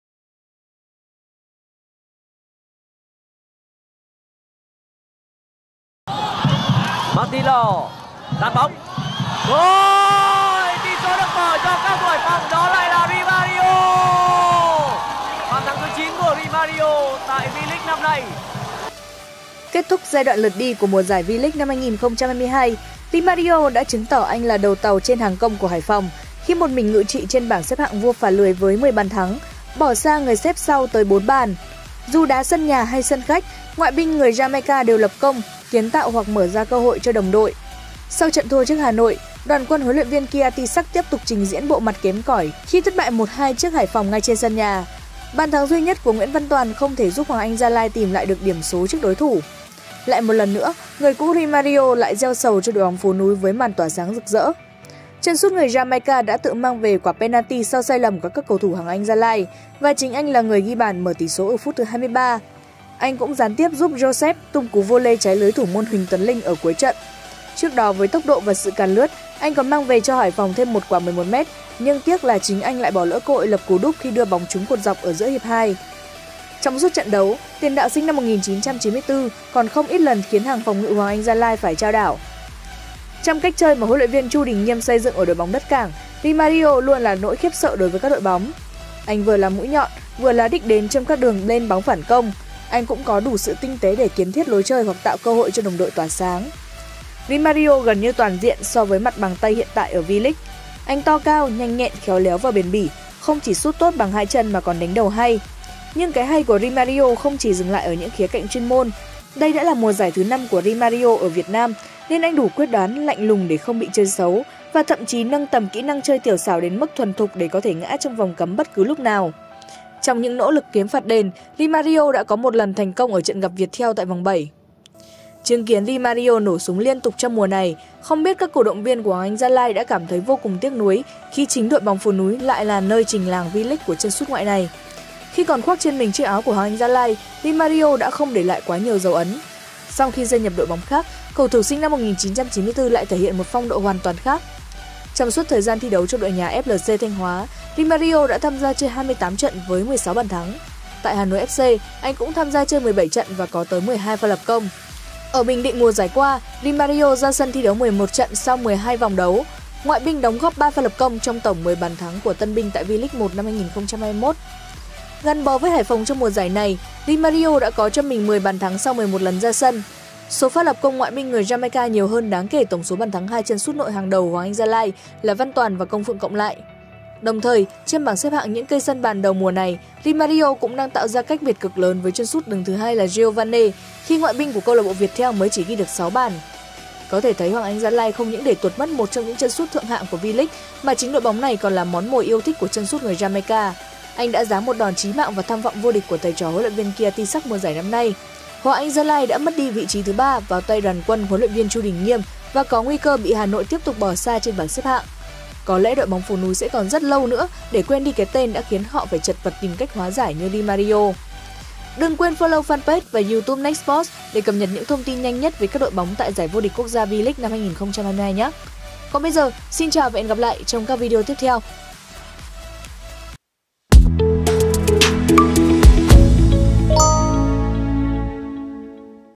Nhìn Rimario ngự trị trên đường đua Vua phá lưới V.League 2022, HAGL có cảm thấy tiếc?
NEXT SPORTS | Nhìn Rimario ngự trị trên đường đua Vua phá lưới V.League 2022, HAGL có cảm thấy tiếc?